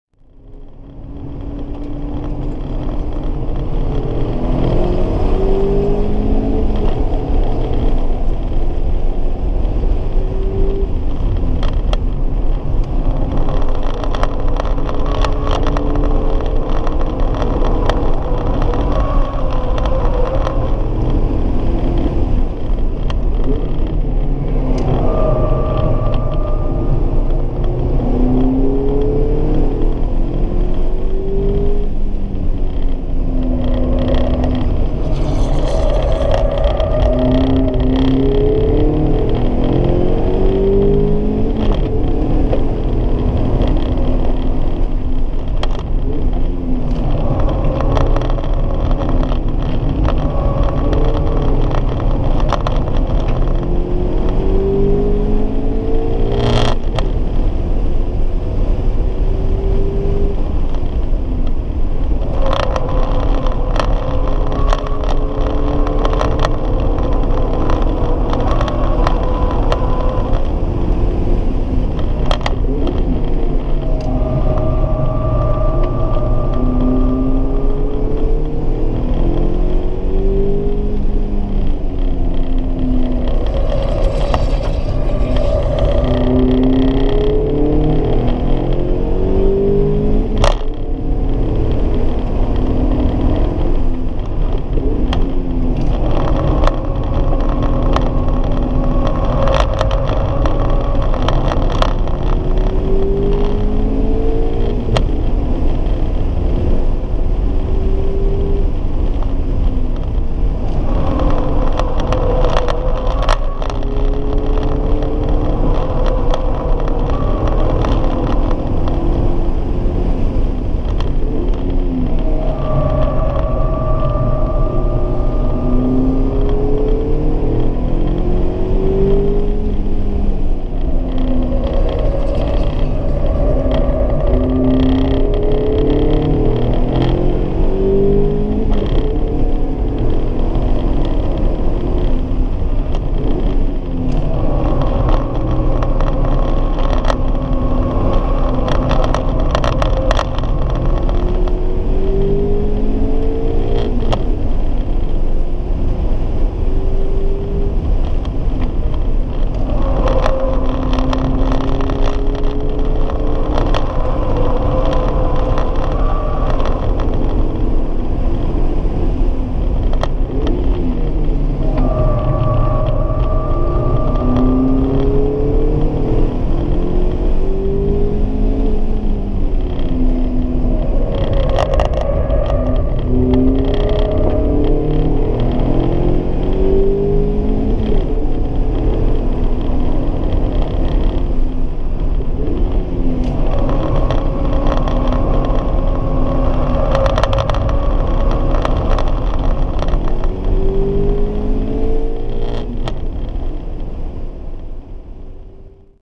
2016-6-5プロクルーズ筑波1000走行会・３本目
タイヤ圧を少し落として、プロドライバーからのアドバイスを受け、今度こそ４８秒切りを目指すべしく走りました(｀・ω・´)

タイムは４８秒５５１。０.０２秒なんて、更新の内に入らんです、ハイ(´・ω・｀)
もう少しラインを煮詰めて、デフを上手く使えれば伸びると思うのですが(^_^;)